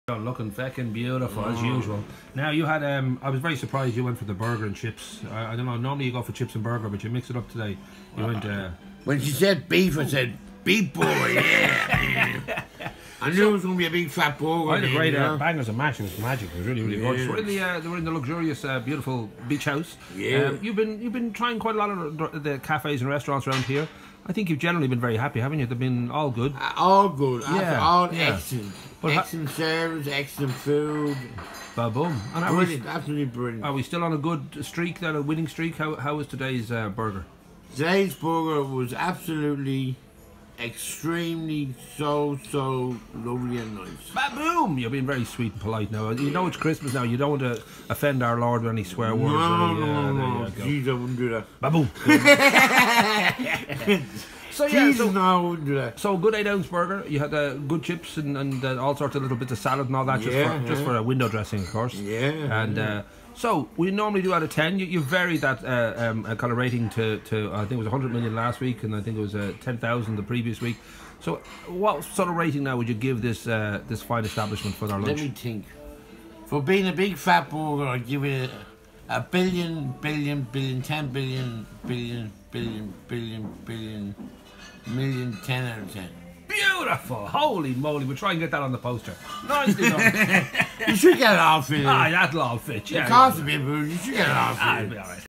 Elvis Vs The Beach House GGTV 22DEC18
One institution meets another as Elvis Alan Presley heads to the harbour, and The Beach House, for - what else? - a great big burger and chips.
But, hey, baby, was this a lunch fit for The King...?